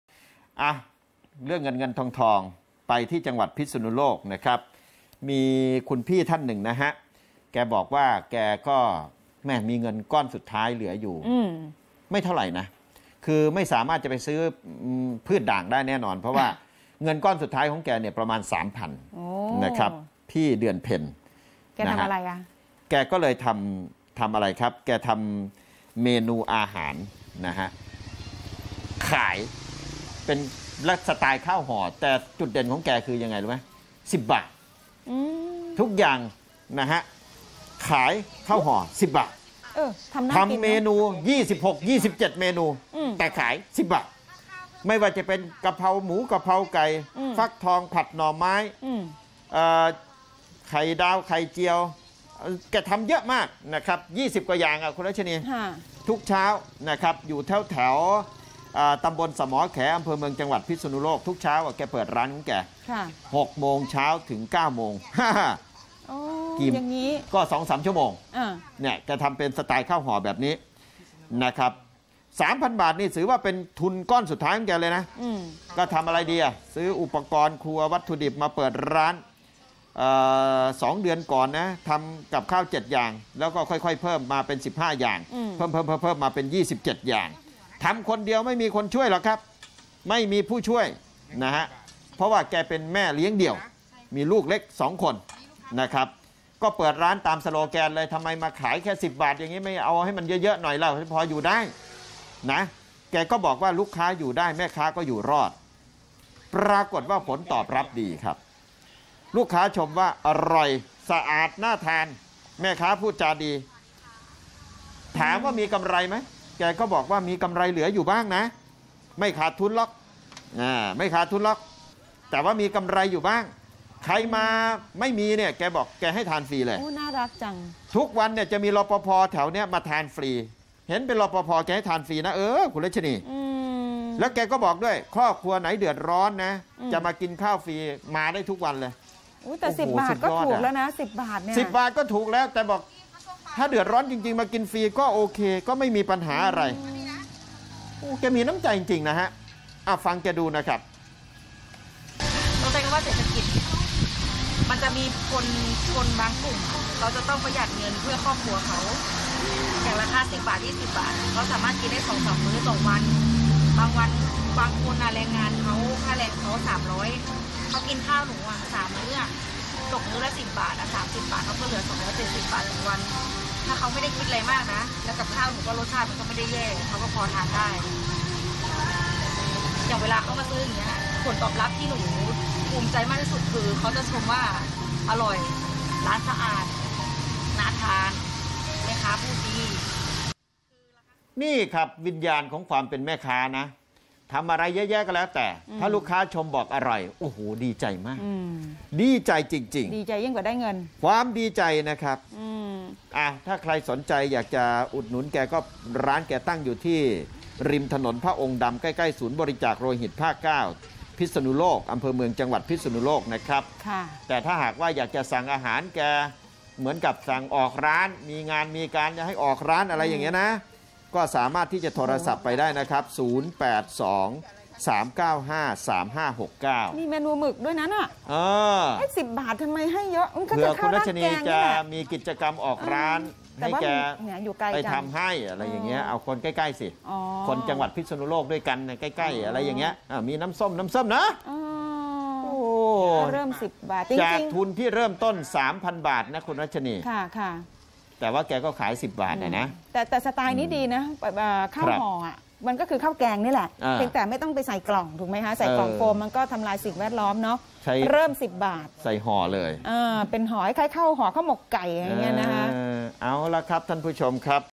สาวพิษณุโลกใช้เงินก้อนสุดท้ายขายข้าวห่อ 10 บาท
แม่ค้าสาวชาวพิษณุโลกใช้เงินลงทุนก้อนสุดท้าย 3,000 บาท เปิดขายข้าวห่อ 10 บาท หลากหลายเมนูในราคาประหยัด ในสโลแกนลูกค้าอยู่รอด แม่ค้าอยู่ได้ เปิดได้ 2 เดือน ลูกค้าขาประจำเพียบ